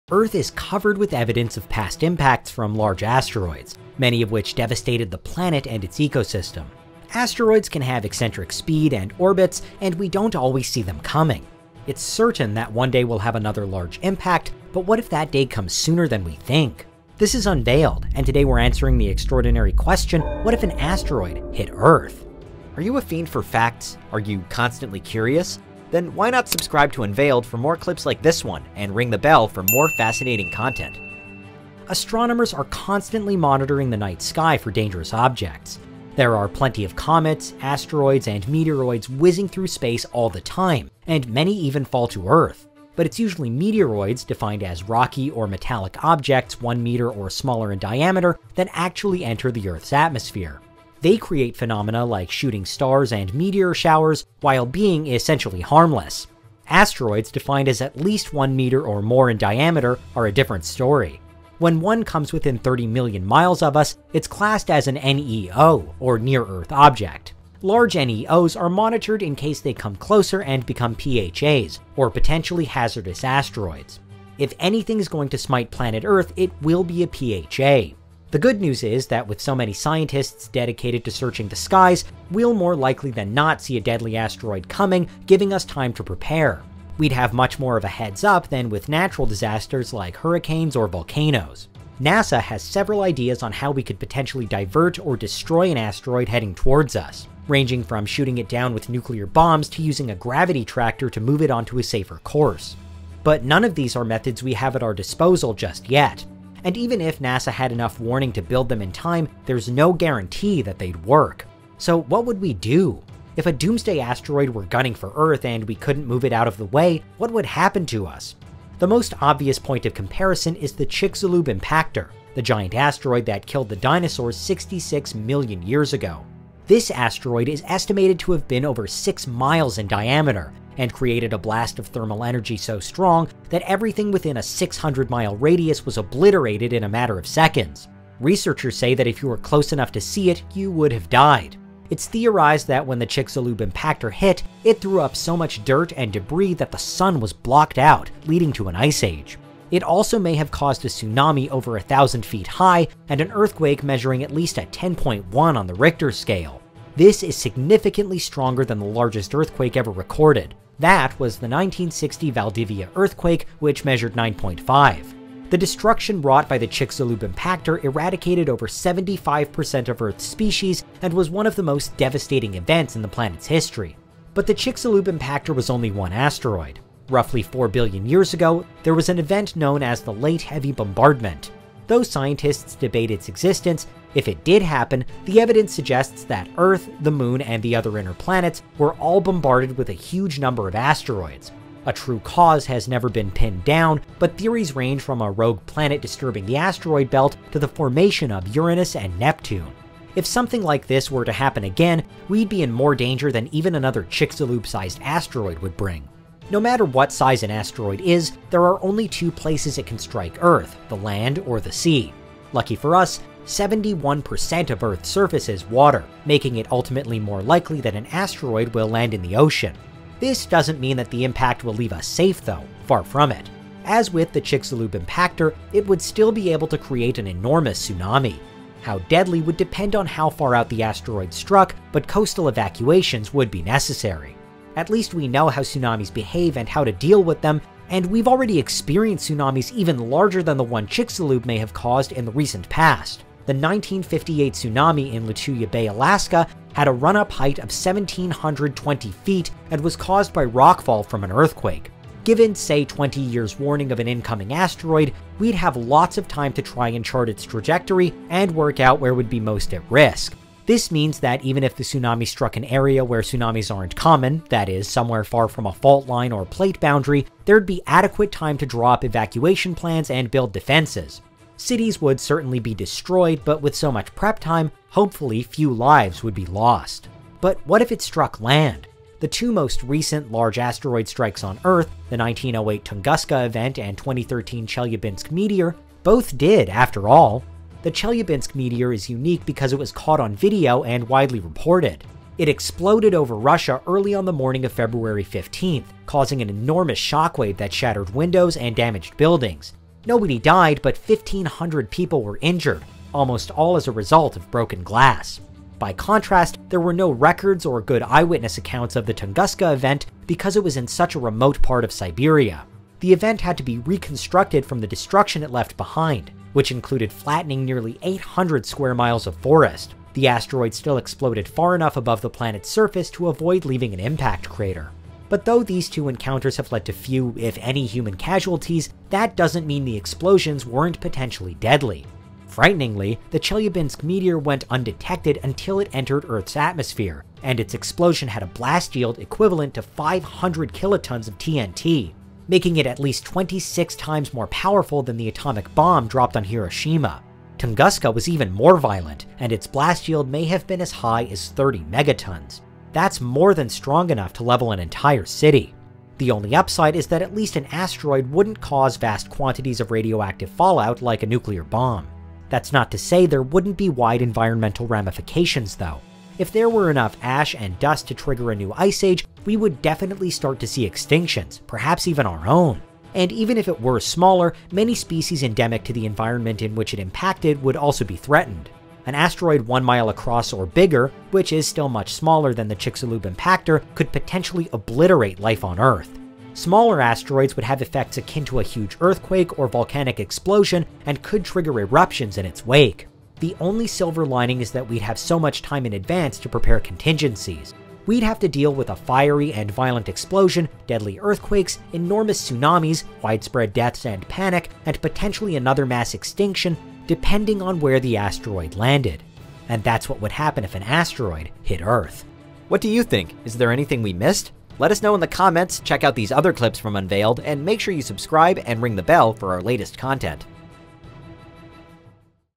What If An Asteroid Hit Earth? | Unveiled
Earth has witnessed massive, asteroid strikes in its past, and will do again... the only questions is "when?"

In this video, Unveiled discovers what would happen if an asteroid struck Earth... Could we survive if a massive space rock smashed into our planet? Or would we be doomed to total extinction?